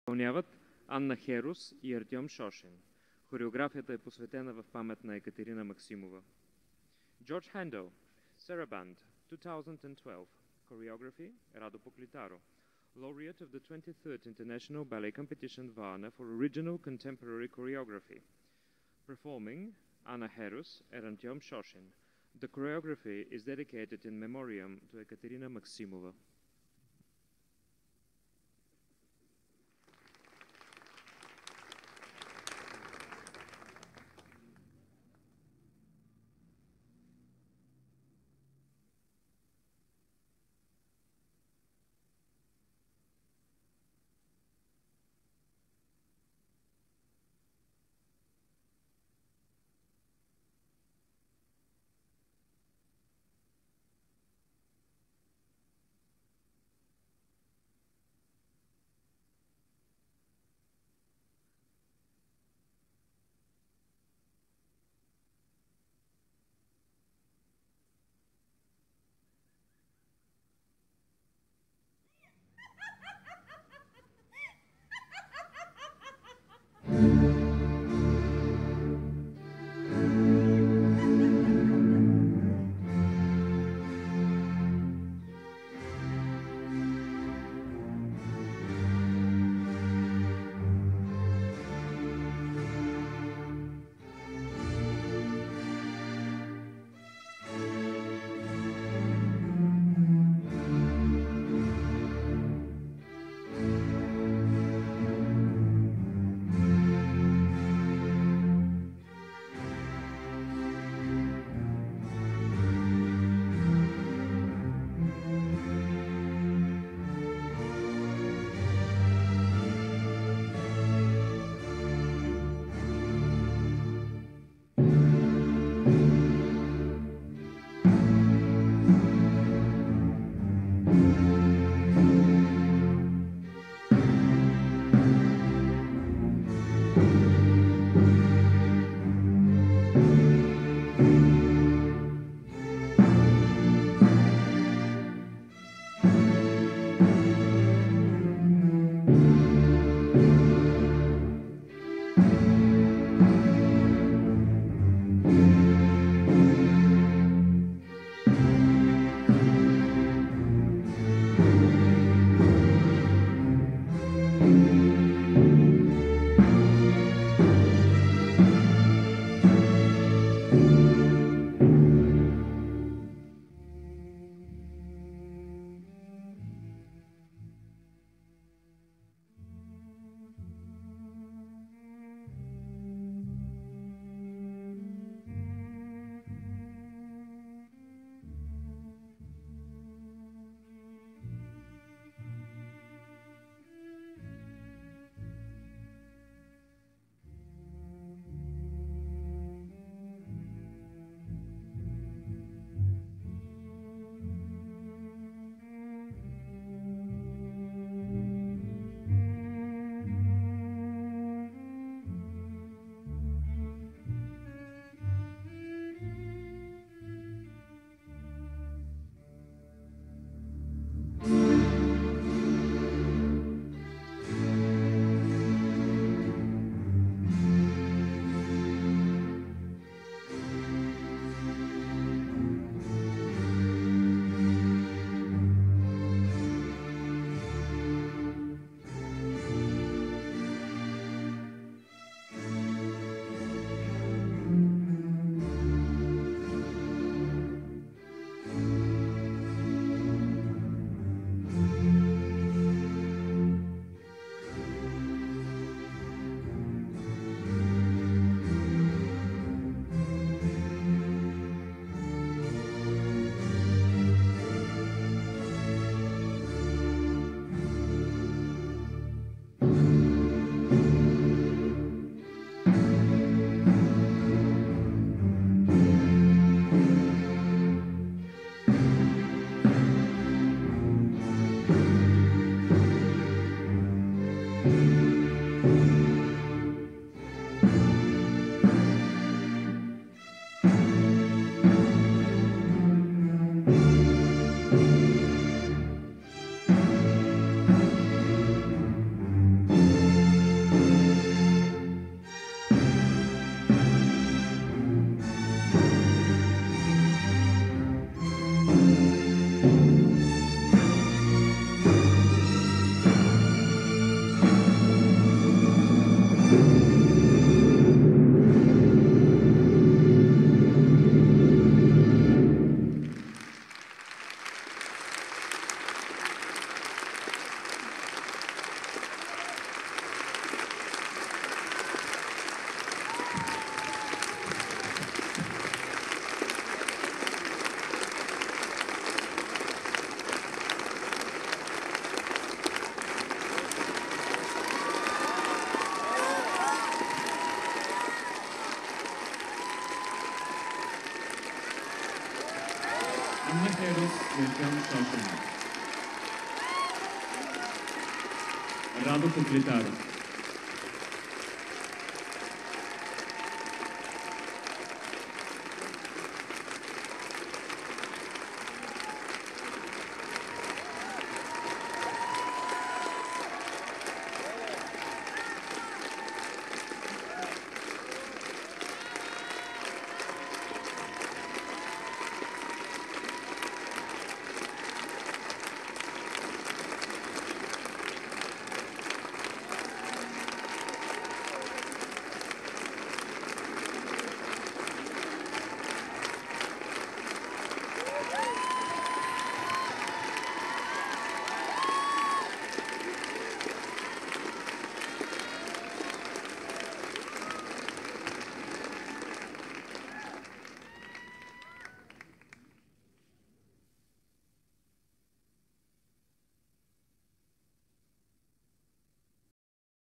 Radu Poklitaru.Anna Gerus, Artem Shoshin "Sarabande" (dadicated to Ekaterina Maximova)Varna 2016
Choreography: Radu Poklitaru
dadicated "in memoriam" to Ekaterina Maximova
Dancers: Anna Gerus / Artem Shoshin